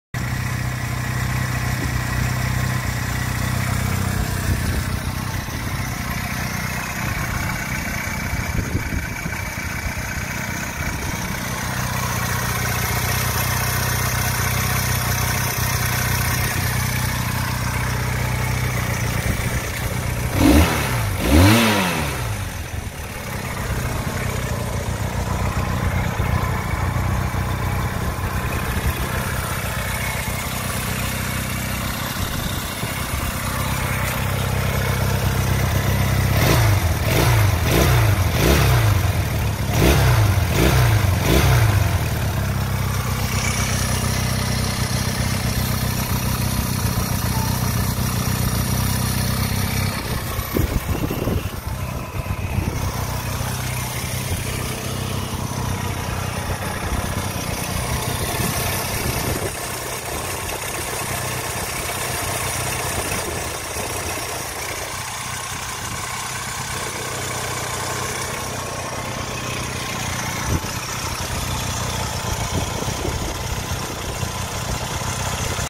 1976 Honda CB200T 1700 Original Miles
1976 Honda CB200T 1700 Original Miles.   Before clean up and refresh to be be put back on the road.